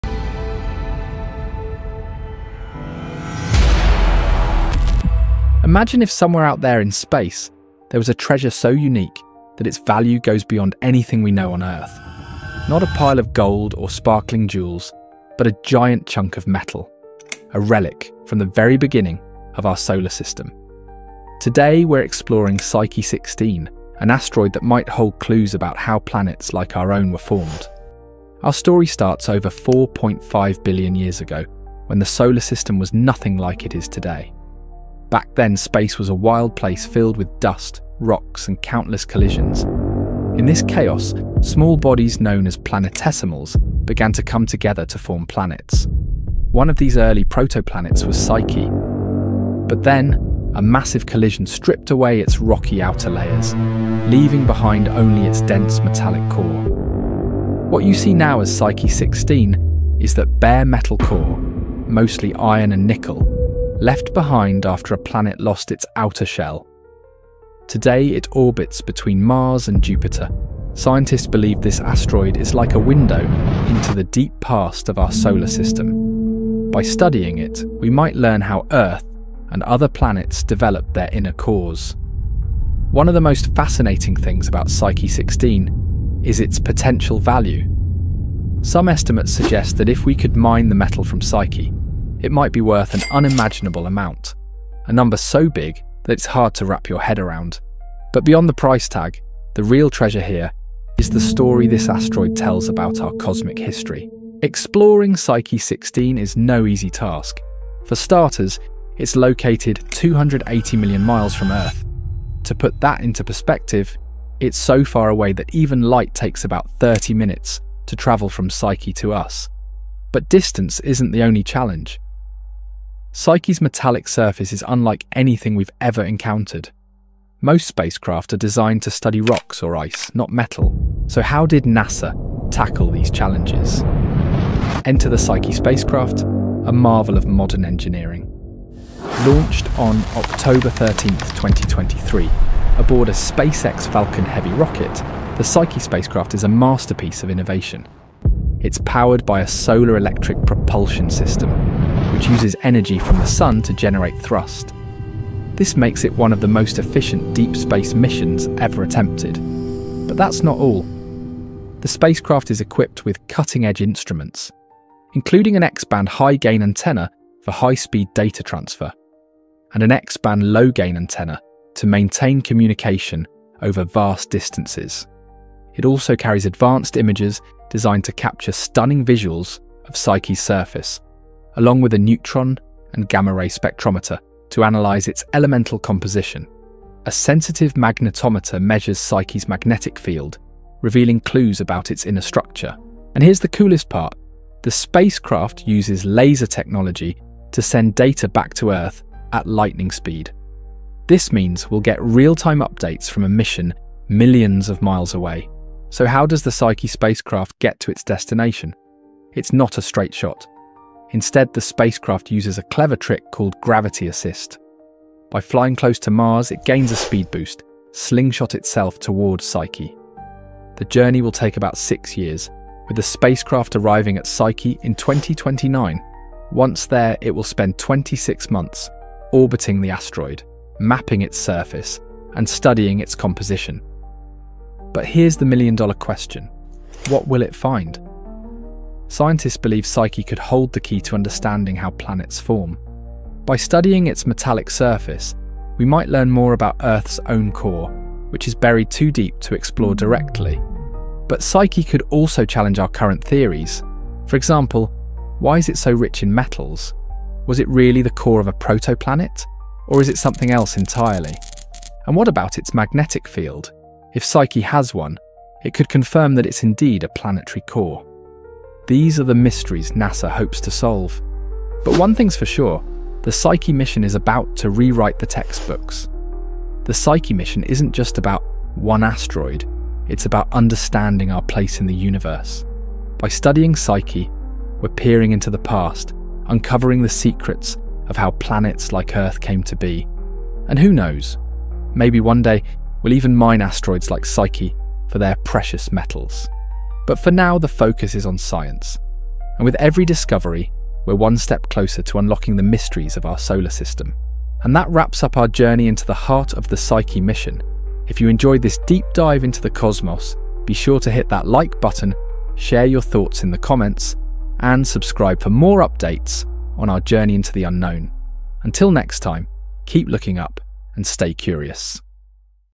Psyche 16: The Most Expensive Object in Our Solar System! 💰
What if an asteroid could make everyone on Earth a billionaire? Meet Psyche 16 – the most valuable object in our solar system, worth a staggering $10,000 quadrillion! But is it really a goldmine, or does it hold secrets about our planet's core? Join us as we explore NASA's daring Psyche Mission and uncover the mysteries of this metallic world. Don’t miss this cosmic adventure!